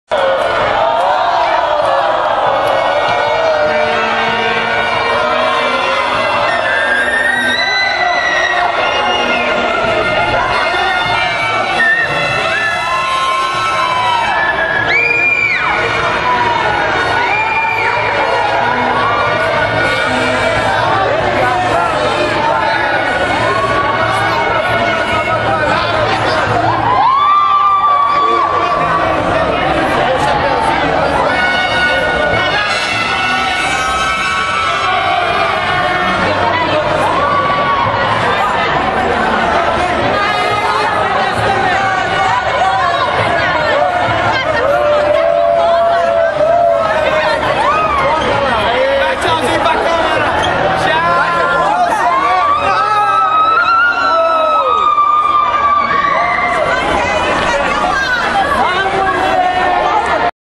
CNEC Além Paraíba Festa Junina 19junho2010 (99) galera.MP4
Forronec Além Paraíba, a maior e melhor festa junina da região.
Galera, momentos antes da entrada da turma para a grande apresentação do forro do "vai quem quer".
19 de junho de 2010